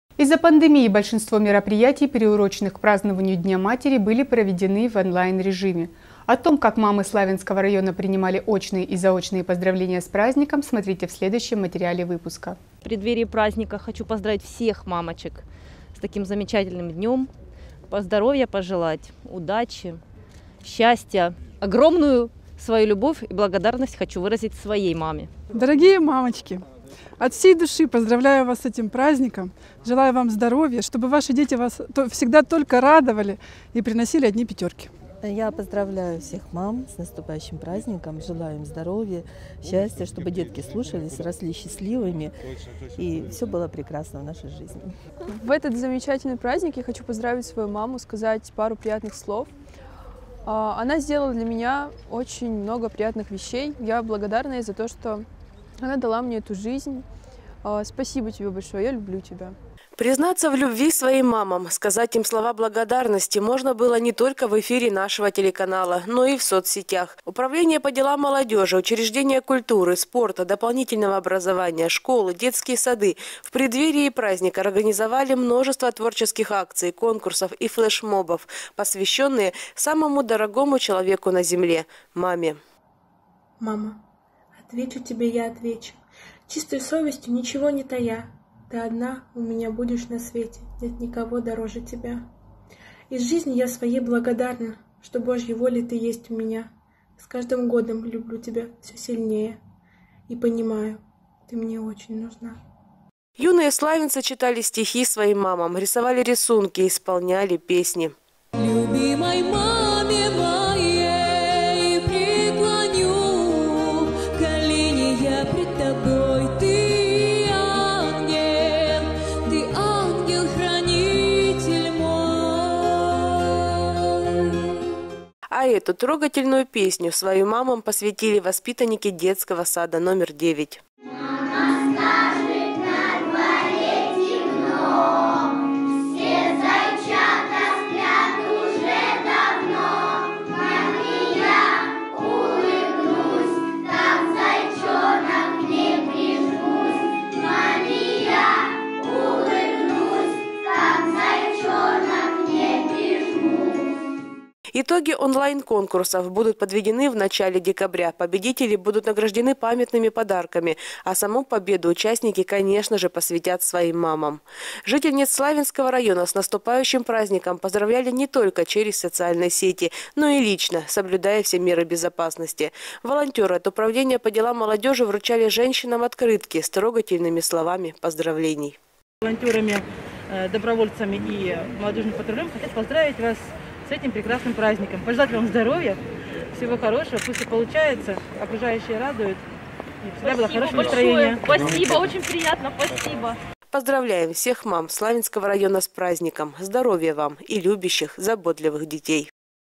Поздравления самым близким и родным
Из-за пандемии большинство мероприятий, приуроченных к празднованию дня матери, были проведены в онлайн режиме. Сюжет о том, как мамы Славянского района принимали очные и заочные поздравления с праздником